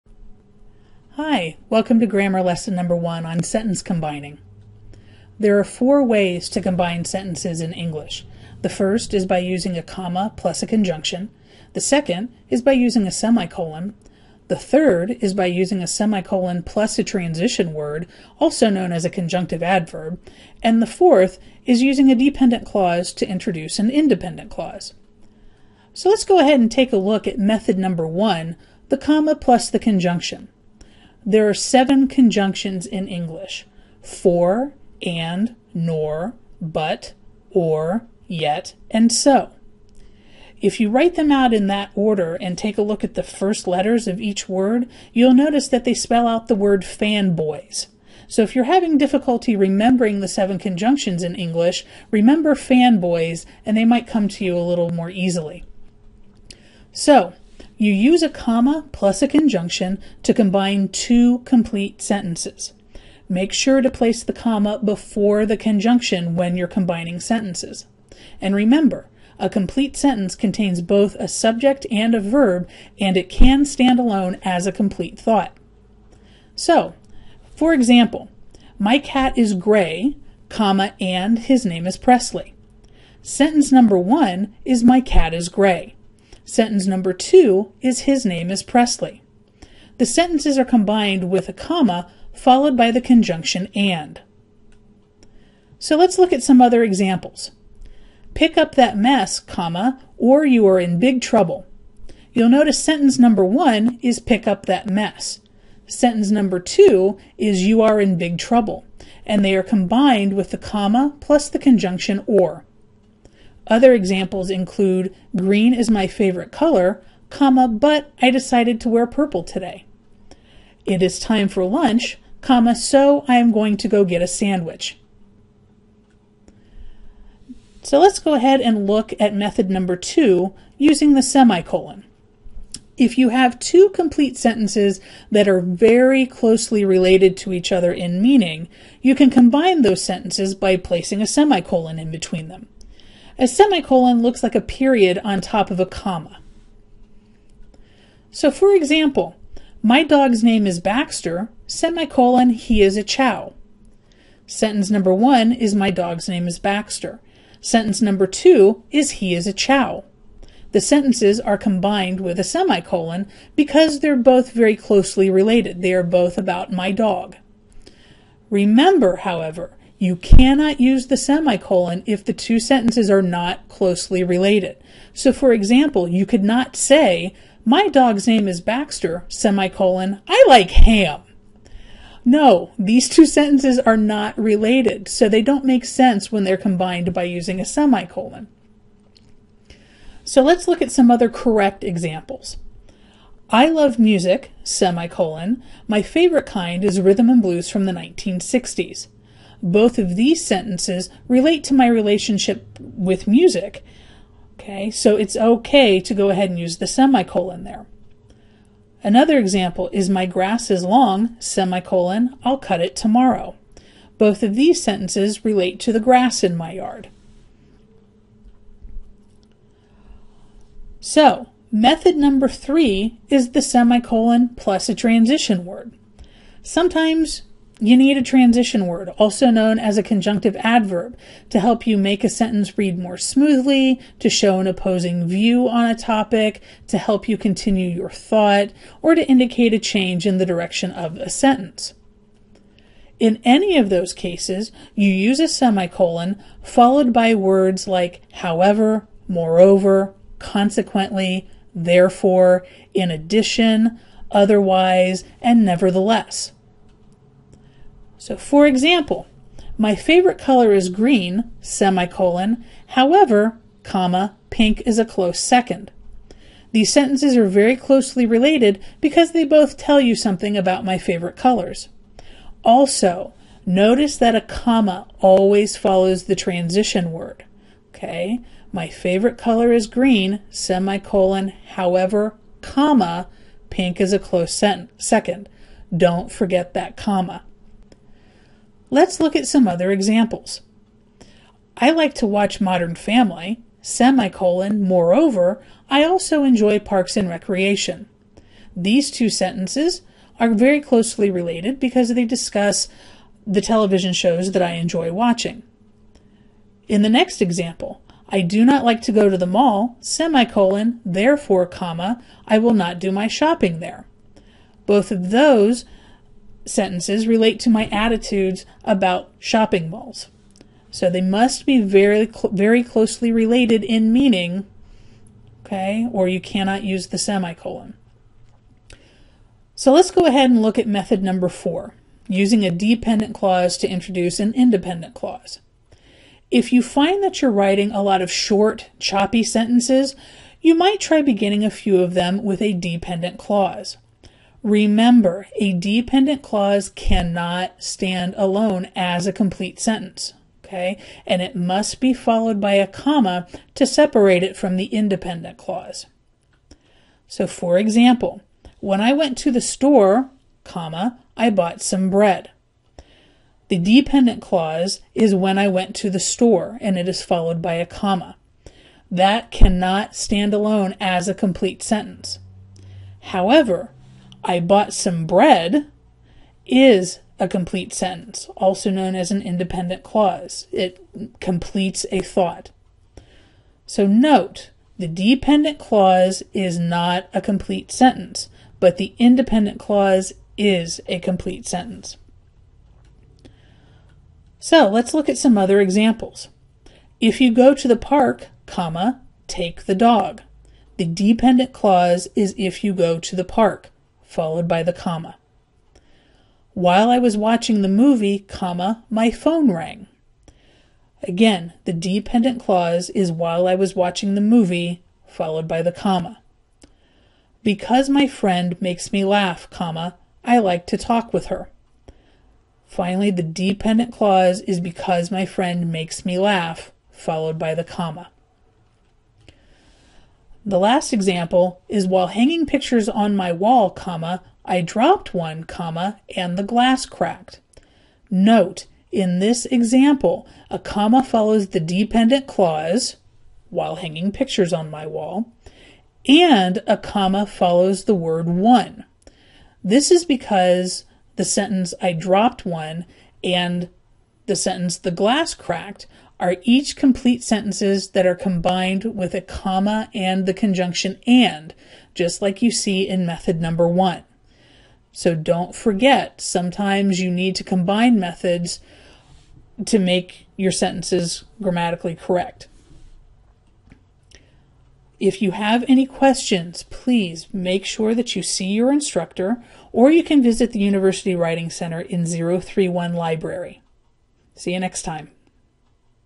Sentence Combining
This video discusses the four ways to combine sentences in English.

Table of Contents:

00:00 - Home
00:25 - Method 1: Comma + Conjunction
00:25 - Method 1: Comma + Conjunction
02:06 - Method 2: Semicolon
03:39 - Method 3: Semicolon + Transition
05:28 - Method 4: Dependent + Independent Clause